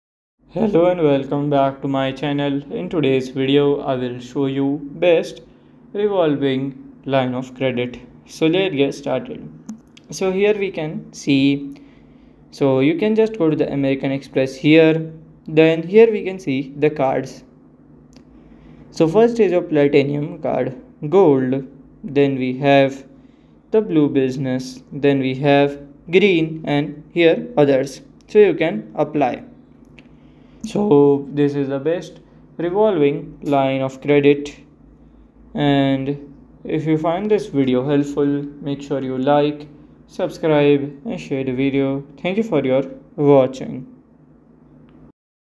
✅ What Is the Best Revolving Line of Credit? (Which Is The Best Revolving Line Of Credit?)
Explore the best revolving lines of credit to manage your cash flow and meet business expenses. This guide will cover top lines of credit, their features, benefits, and application process.

Key Learning Points:
• Top revolving lines of credit
• Key features and benefits
• Application process
• Comparison of leading credit lines
• Tips for managing cash flow